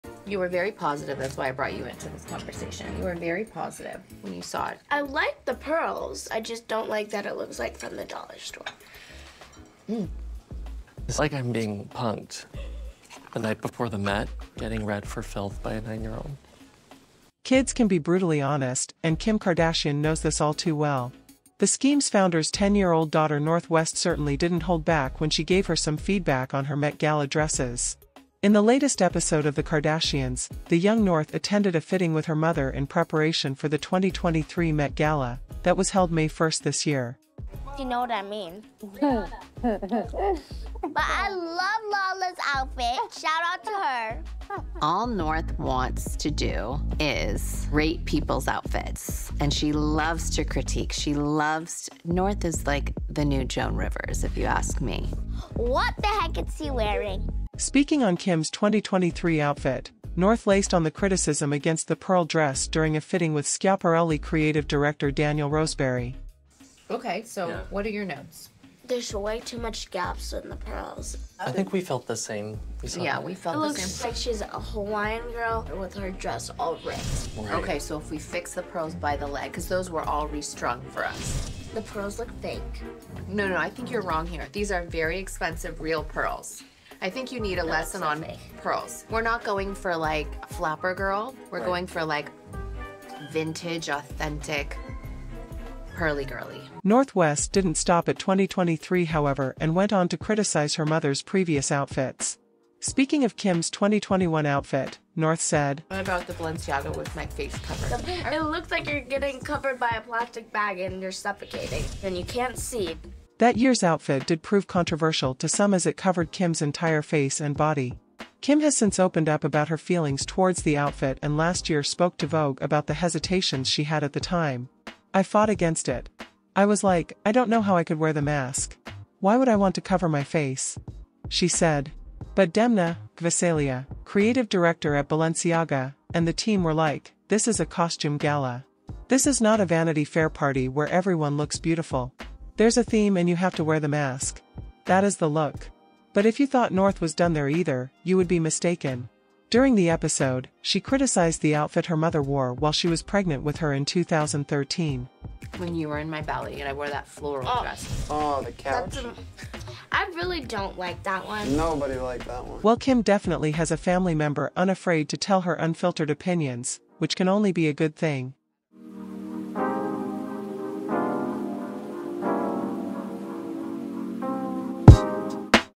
North West Brutally DRAGS Kim Kardashian!!! | The Kardashians
Today We Have North West Kim Kardashians Eldest Daughter Who Has Roasted Her Moms Met Gala Dresses Calling Them Fake & Cheap

👍Drop a LIKE for MORE Trending Topics